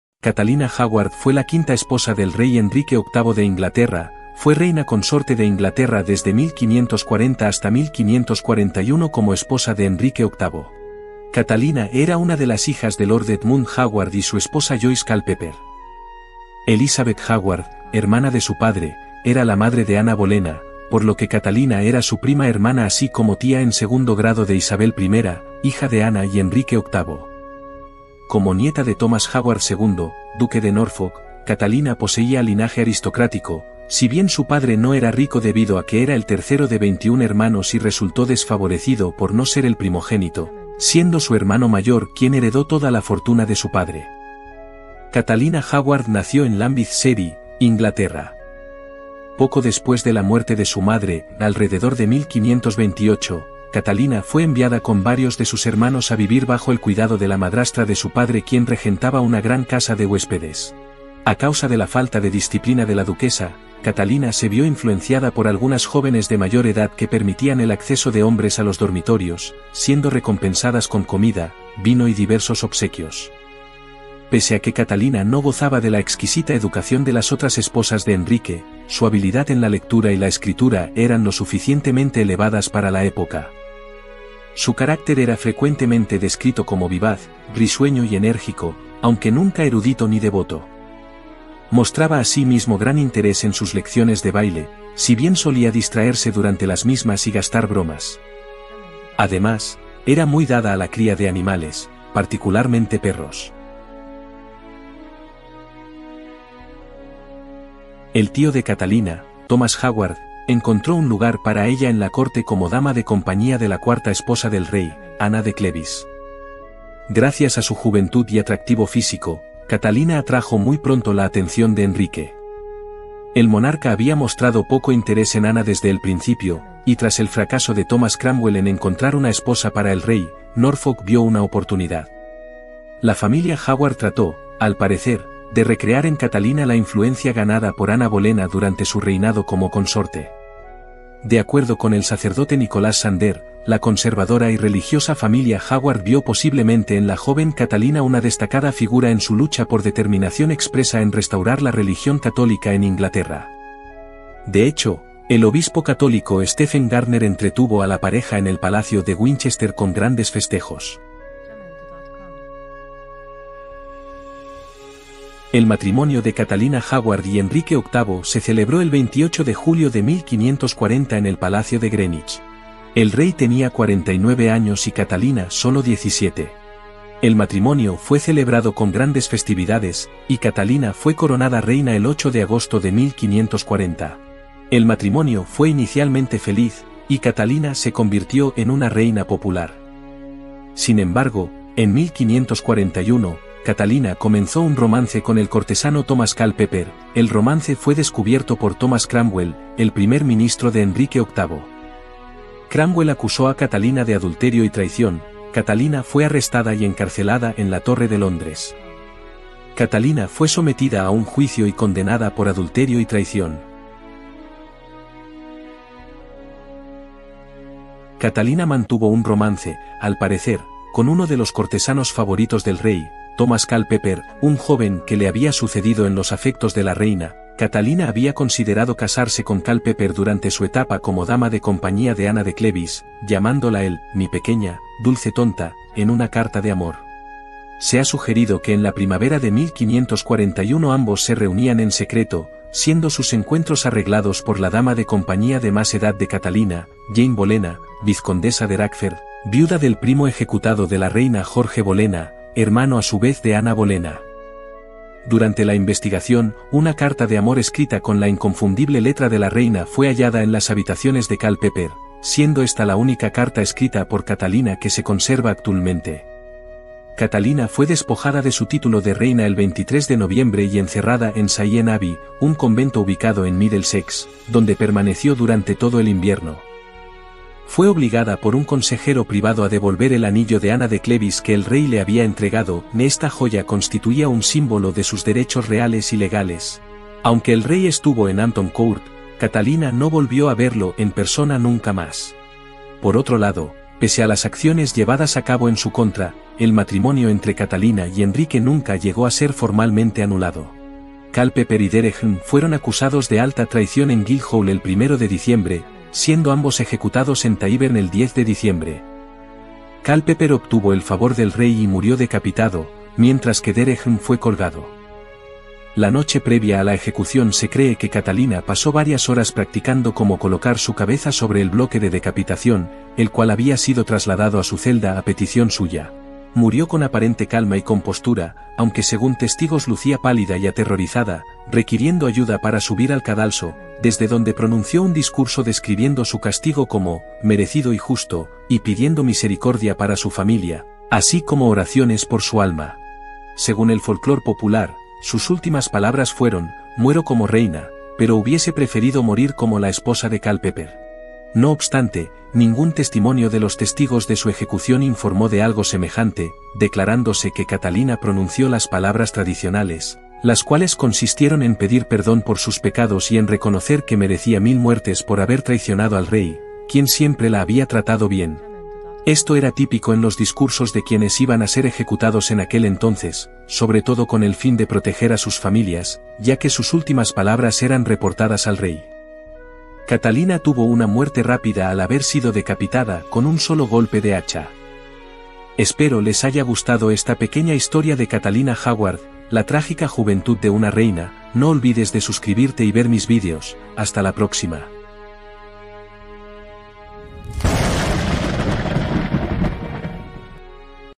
¿Quién fue Catalina Howard? La Trágica Juventud de una Reina
Catalina Howard fue la quinta esposa del rey Enrique VIII de Inglaterra. fue reina consorte de Inglaterra desde 1540 hasta 1541 como esposa de Enrique VIII.
Catalina era una de las hijas de Lord Edmund Howard y su esposa Joyce Culpeper. Elizabeth Howard, hermana de su padre, era la madre de Ana Bolena, por lo que Catalina era su prima hermana así como tía en segundo grado de Isabel I, hija de Ana y Enrique.